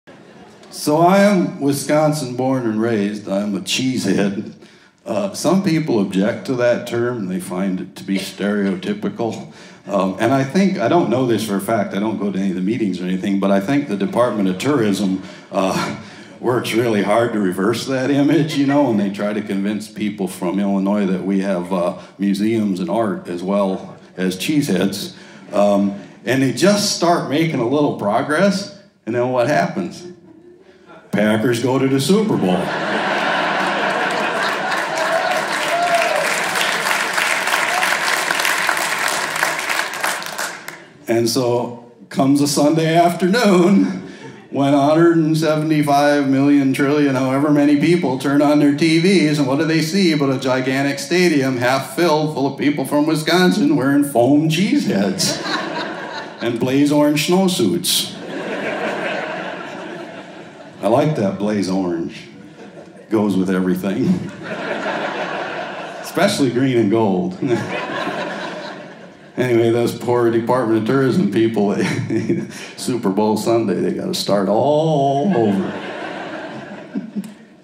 Green and Gold and Blaze Orange
Not so long ago, author, humorist and amateur pig farmer Michael Perry fed the chickens, changed his boots, kissed his wife and daughters, and drove downstate to a refurbished opera house in Stoughton, Wisconsin. Waiting for him onstage was a microphone; waiting in the seats was a sold-out house full of folks ready to laugh. For the next two hours the walls of the old opera hall echoed with stories of love and long underwear, happily failed bachelors, aggressive roosters, one-eyed joke-telling butchers, the glories of pickup-truck-based courtship…and wave after wave of laughter. And then – right at the end there – came the story of the little girl, the guinea pig, and a math lesson gone wrong.